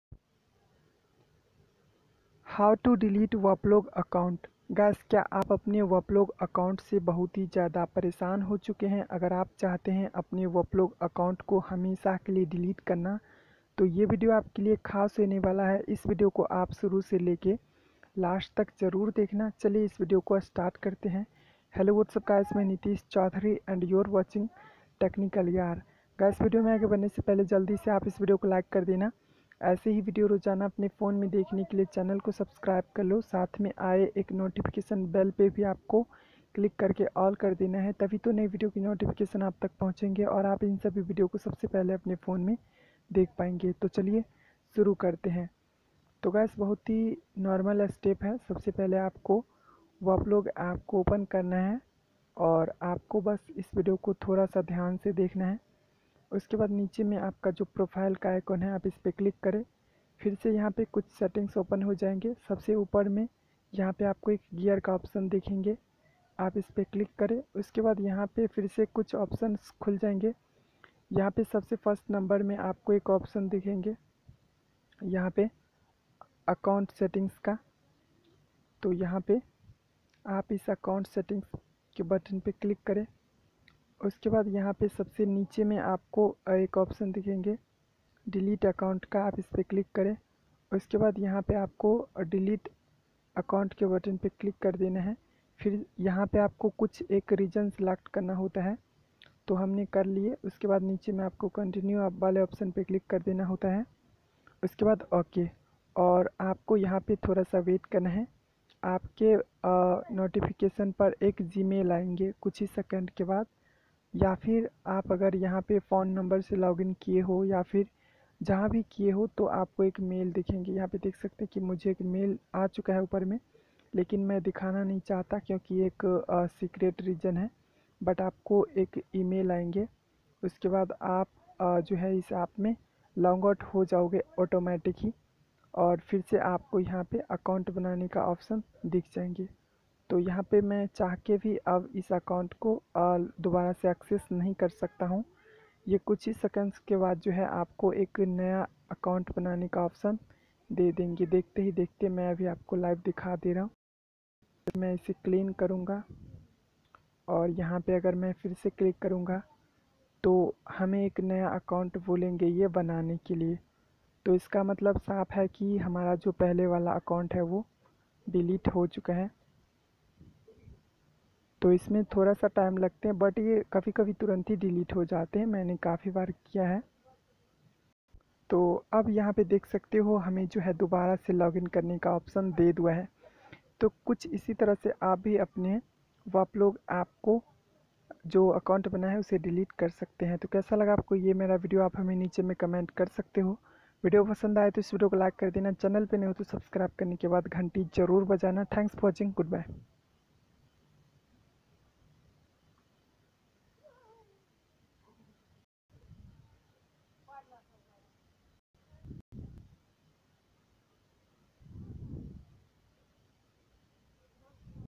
waplog account delete kaise kare || waplog app me account delete kaise kare || how to delete waplog
🔶Video Description➡iss video me my aap Sabhi logon ko bataya hun waplog account delete kaise kare sabse pahle Hakuna aap ko open Kare aur my ID per click Kare FIR aapko chatting ke button per click kar dena hai aapko Ek legal ka option Milenge use per click Kare aur FIR sabse Neeche me delete my account ke button per click Karen aur ok Kare aap ka hakuna account permanently delete ho jaenge

💠Video editing and voice by - Nitish Chaudhary